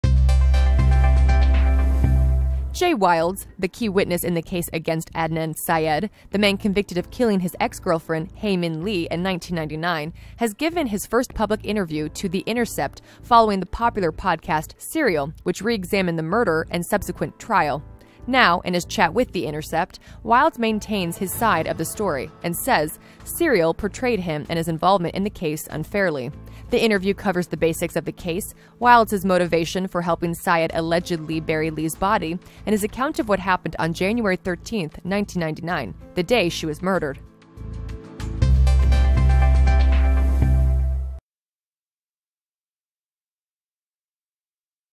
'Serial' Witness Jay Wilds Opens Up for First Time
Jay Wilds, the key witness in the case against Adnan Syed — the man convicted of killing his ex-girlfriend Hae Min Lee in 1999 — has given his first public interview to The Intercept following the popular podcast, "Serial," which reexamined the murder and subsequent trial.
Now, in his chat with The Intercept, Wilds maintains his side of the story, and says "Serial" portrayed him and his involvement in the case unfairly. The interview covers the basics of the case, Wilds' motivation for helping Syed allegedly bury Lee's body and his account of what happened on January 13th, 1999, the day she was murdered.